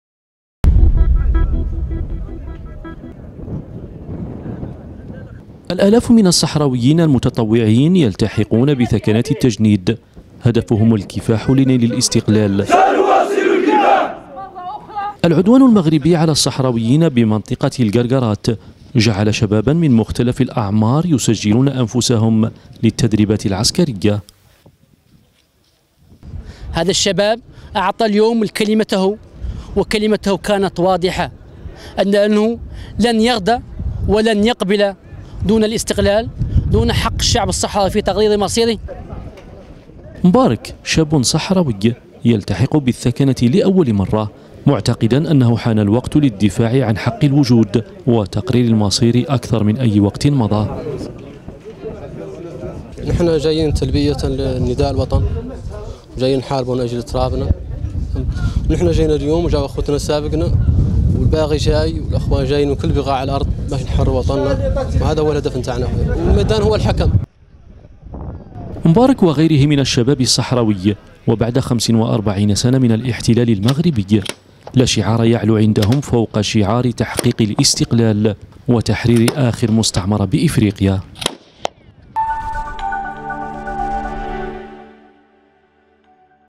الكفاح المسلح.. سبيل الشباب الصحراوي للاستقلال
مخيمات اللاجئين الصحراويين (وأج)- تستقبل مراكز التدريب التابعة للجيش الصحراوي العديد من الشباب الذين يحلمون باستقلال أرضهم من المحتل المغربي، ويعتقدون أن الكفاح المسلح هو الوسيلة الأنجع لنيل الحرية.